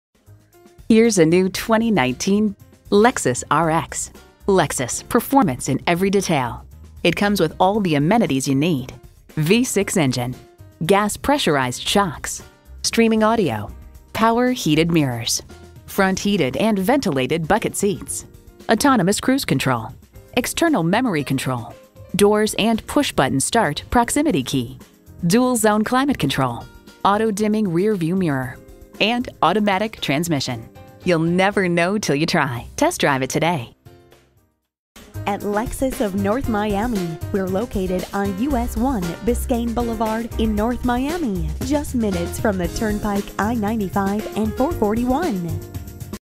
New 2019 Lexus RX Miami, FL #9351700
Year: 2019
Make: Lexus
Model: RX
Trim: 350
Engine: 3.5L V6 DOHC VVT-i 24V
Transmission: 8-Speed Automatic
Color: Silver
Interior: Blk Lth
Stock #: 9351700
VIN: 2T2ZZMCA0KC136593
Silver Lining Metallic 2019 Lexus RX 350 FWD 8-Speed Automatic 3.5L V6 DOHC VVT-i 24V Blk Lth.brbrRecent Arrival! 20/28 City/Highway MPG

Address:
14100 Biscayne Blvd